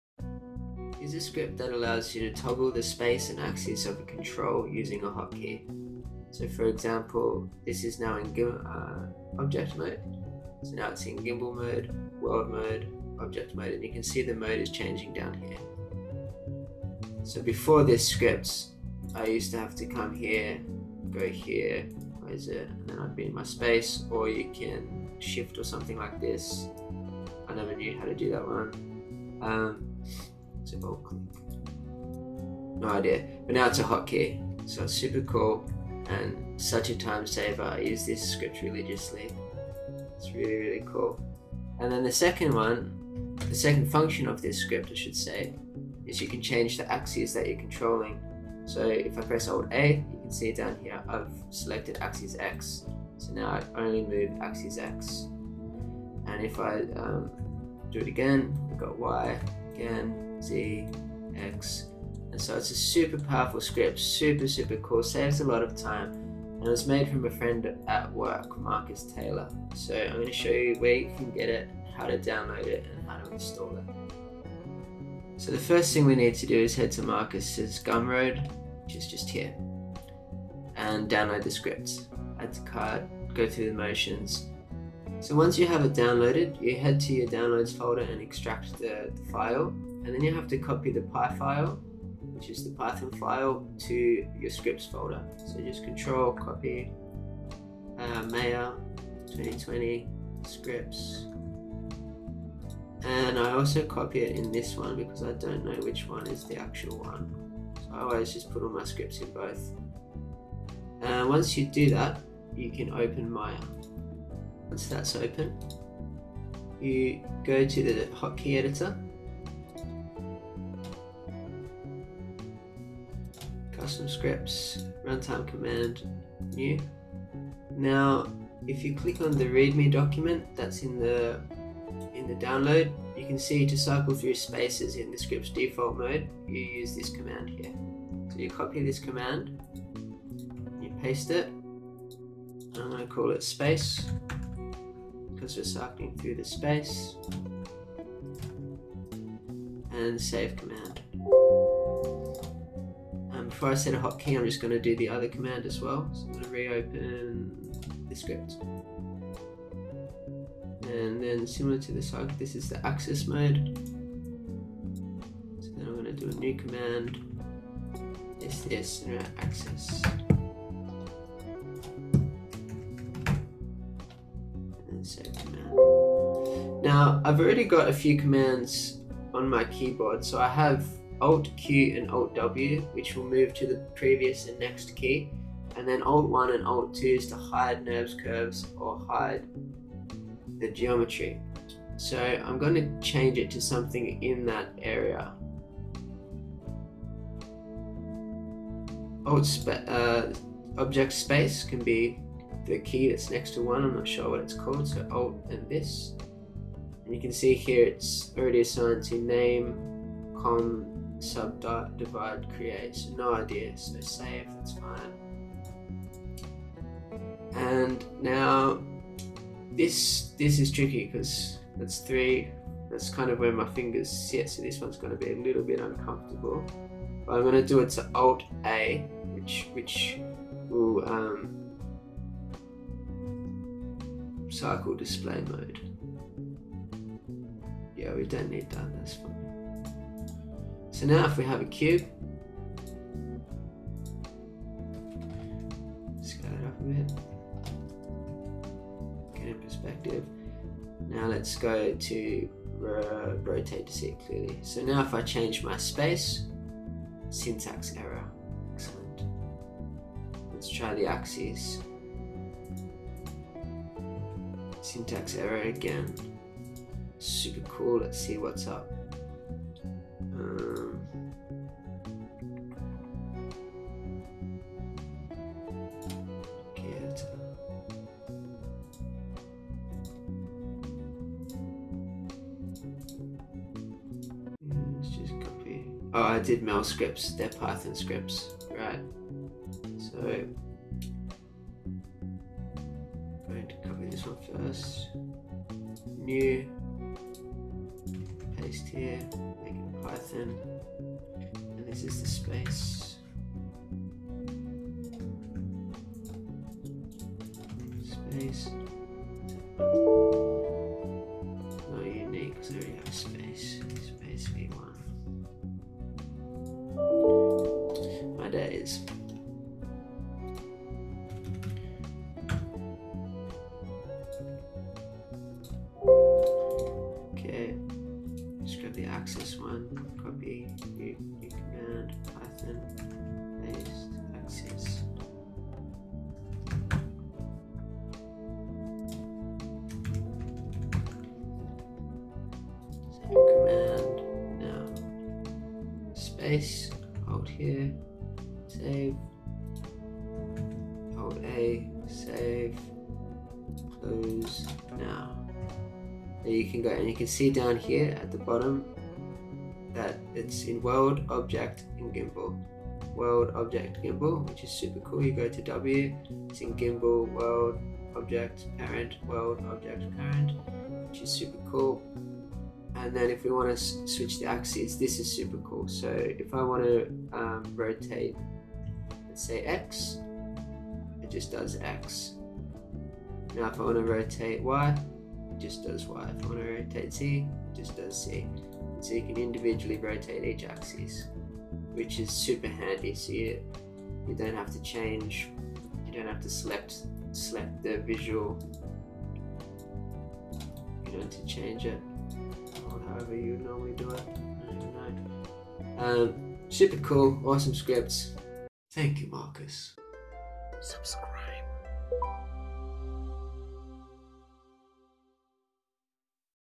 How to Toggle Object Space and Axis | Maya Script | Hotkey
The script we have all been waiting for - mmManipSpaceCycle - Version 4.00  by Marcus Taylor.

This script is designed to allow for easy cycling through spaces or axis' on Maya's manipulators.
With the ability to fully customize each function as you like, this script should tailor to a wide variety of workflows.

To cycle through SPACES in the script's default mode, these two lines will run the space mode either in a hotkey or button:
import mmManipCycle
mmManipCycle.space()

Similarly, to cycle through manipulator AXIS' you can use the axis mode below
import mmManipCycle
mmManipCycle.axis()

Online Animation School specializing in advanced character animation mentorships and beginner courses.

The recruiting company for animators